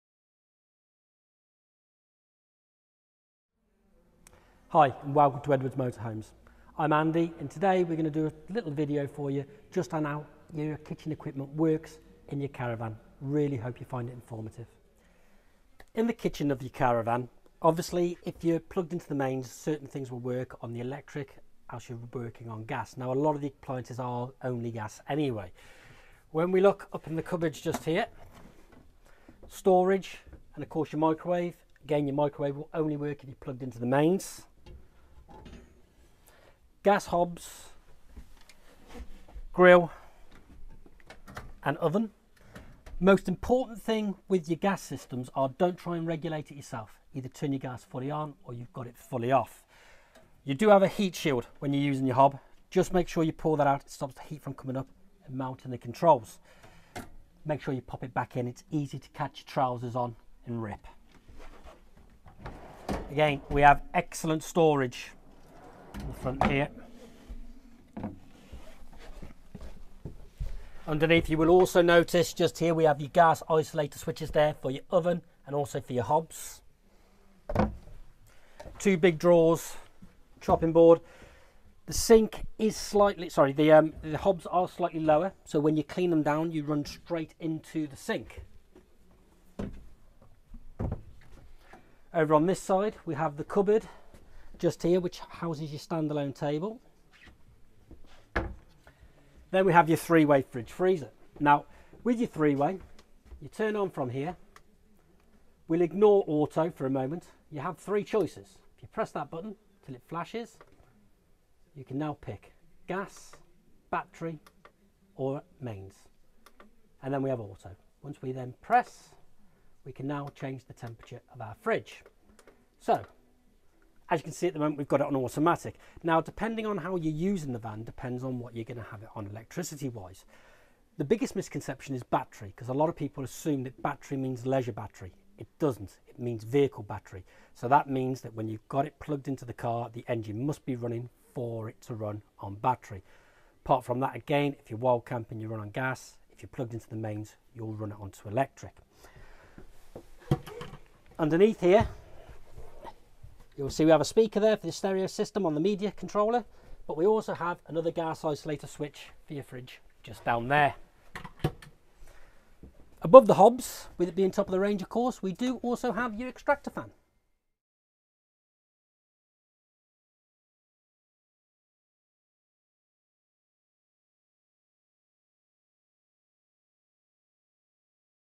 How your caravan kitchen works
Our motorhome and caravan sales manager Andy Dimmock shows us around this Adria Alpina Colorado caravans kitchen and shows us how the appliances work.